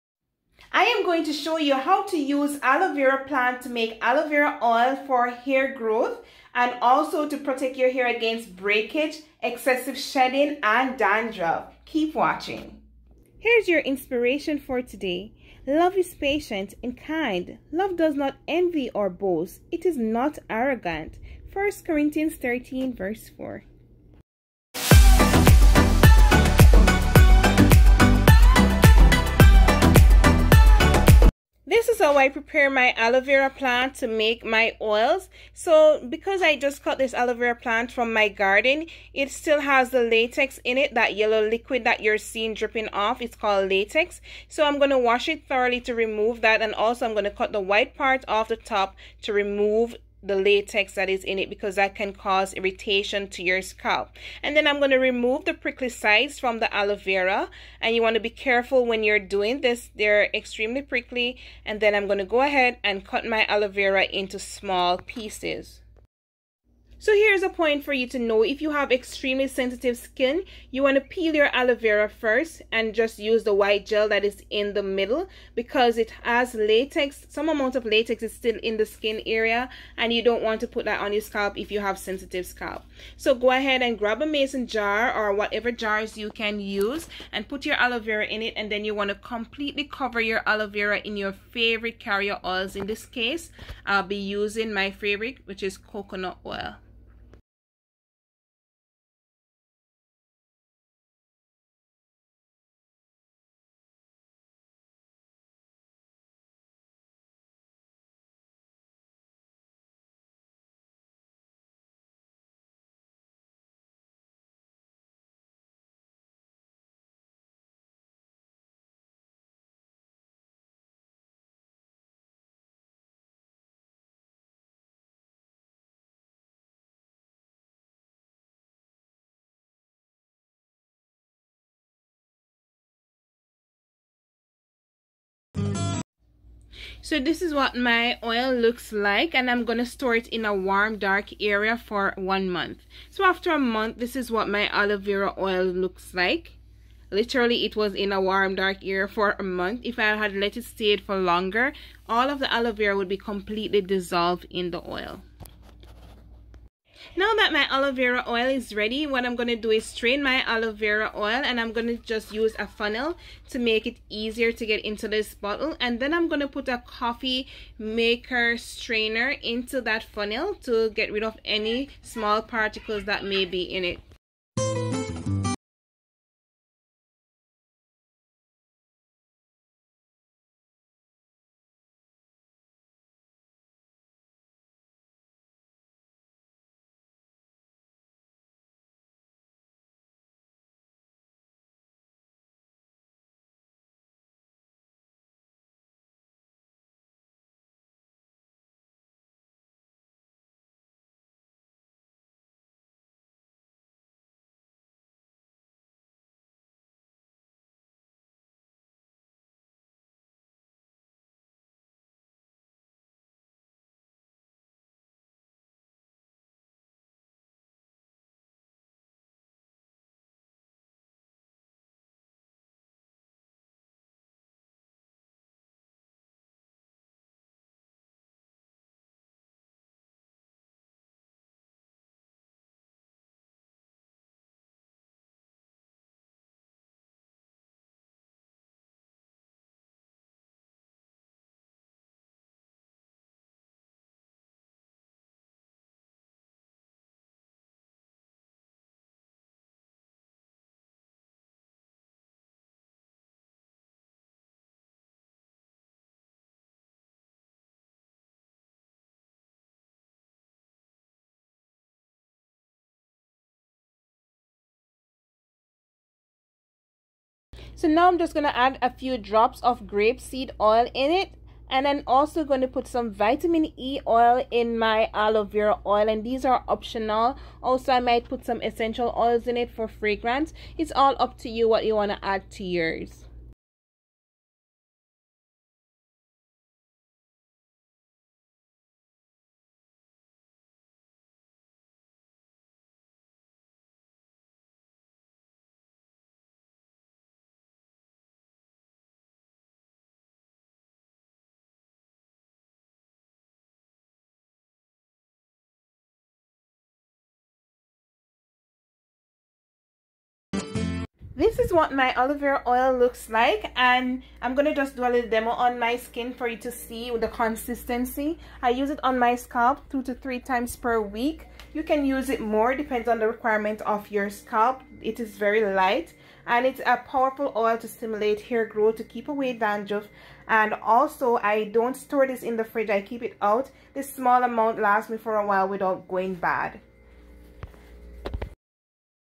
How to make Aloe vera oil for extreme hair growth/diy aloe vera oil for hair lost and Dandruff.
Hey Queen, in this video  I will  show you how I  make my aloe vera oil for extreme  hair growth , hair loss and ,Dandruff.  I show you how I safely prepare my aloe vera  plant by getting rid of all the latex which is the yellow liquid in the aloe vera plant which may cause irritation to your skin if you have  sensitive skin. Aloe vera  plant is extremely beneficial to the scalp and hair. it helps to  promote hair growth because of the the nutrients and enzymes, this is a great reason for you to include aloe vera your hair growth regimen.